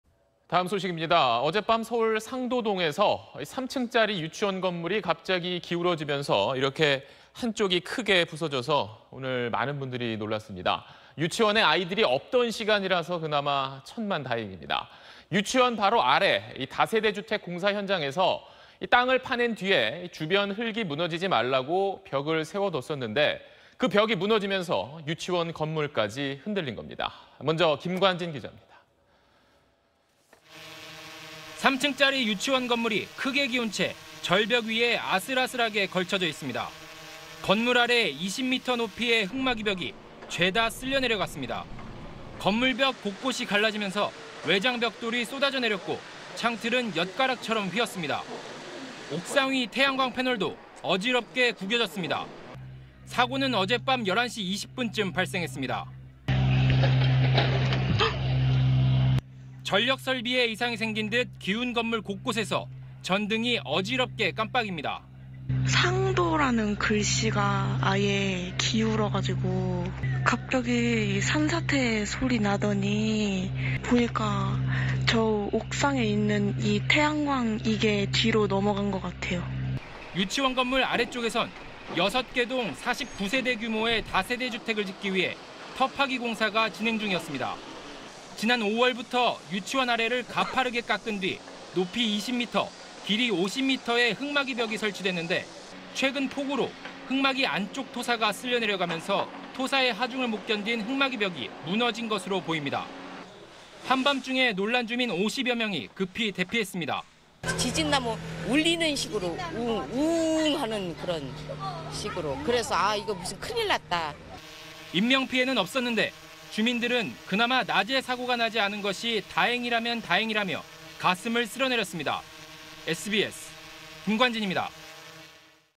굉음과 함께 유치원 '기우뚱'…절벽 위 위태로운 모습 / SBS
어젯(6일)밤 서울 상도동에서 3층짜리 유치원 건물이 갑자기 기울어지면서 이렇게 한쪽이 크게 부서져서 오늘 많은 분들이 놀랐습니다. 유치원에 아이들이 없던 시간이라서 그나마 천만다행입니다. 유치원 바로 아래 다세대 주택 공사 현장에서 땅을 파낸 뒤에 주변 흙이 무너지지 말라고 벽을 세워뒀었는데 그 벽이 무너지면서 유치원 건물까지 흔들린 겁니다.